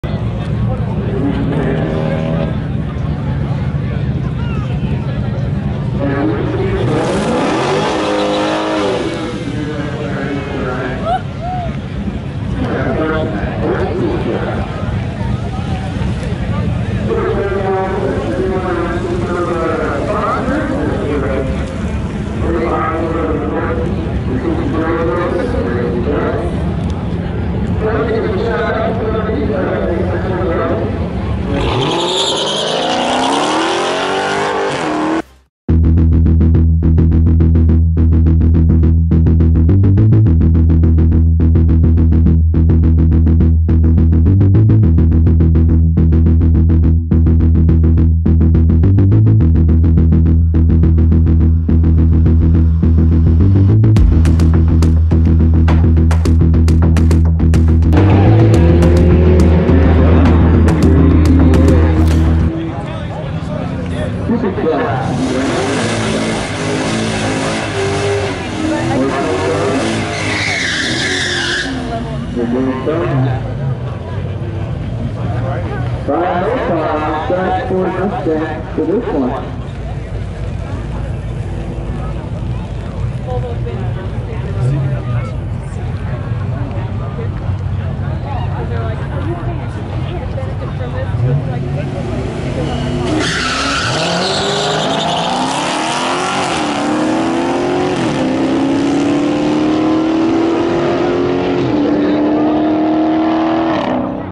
Yellow Daytona 392 (@Bananaboat_392)🍌
Demonic Car Club Member😈

Dodge Charger Daytona 392 with a 6.4L Engine 🍌

Barona Drag Strip